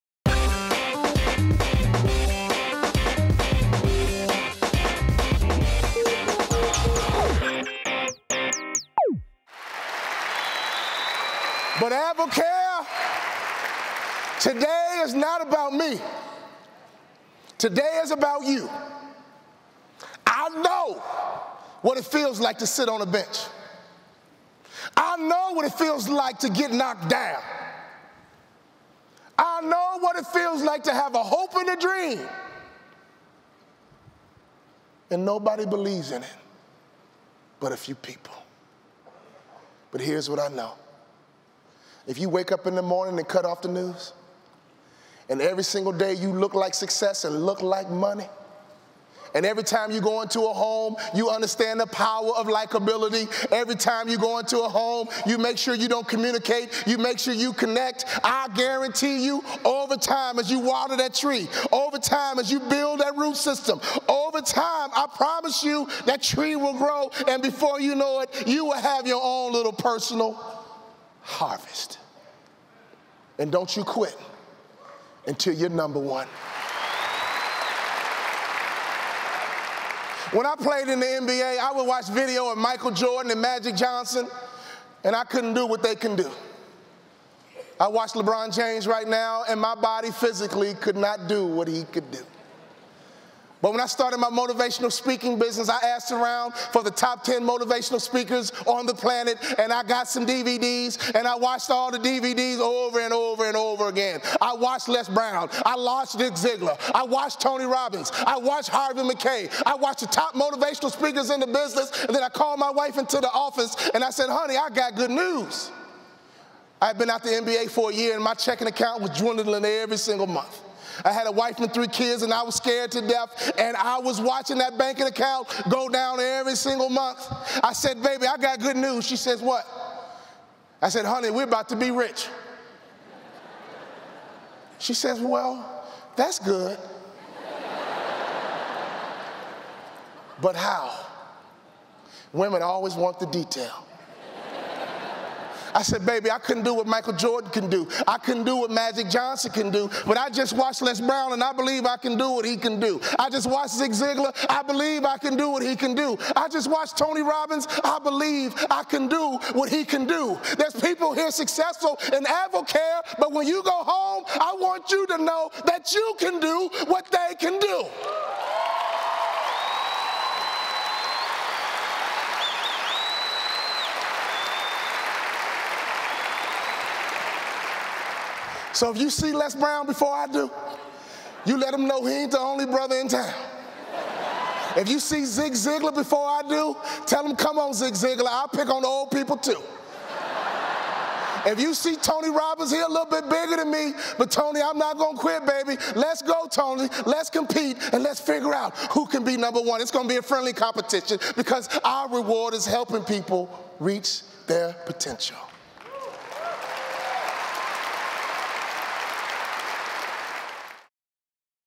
Today Is About You - Walter Bond
Be Your Potential with Walter Bond - Walter discusses what it takes to become #1 in your niche.